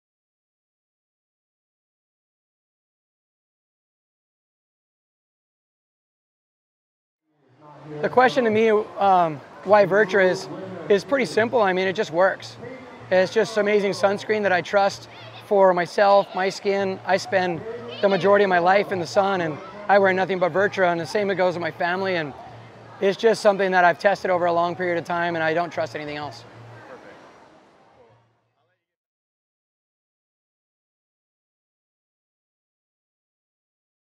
Shane Dorian Vertra Sunscreen Testimonial
Professional surfer and all around waterman Shane Dorian explains why he chooses Vertra Sunscreen.